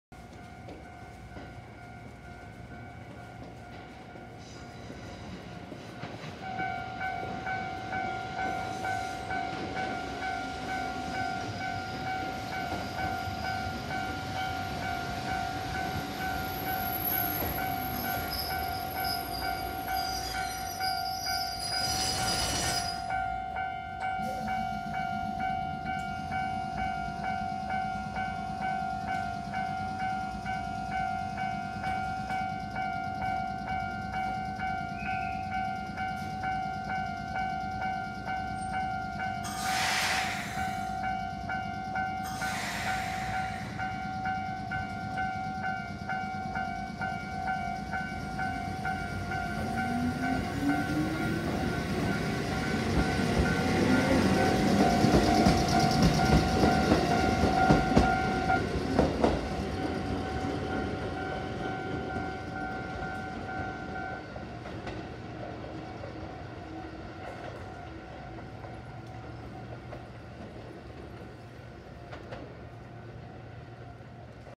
2018.11.30(金)16:32 高松琴平電鉄 1200形1207F(琴平線片原町駅)
もと京急700形の琴電琴平行です。
温暖な高松に似合うカラーリングだと思います。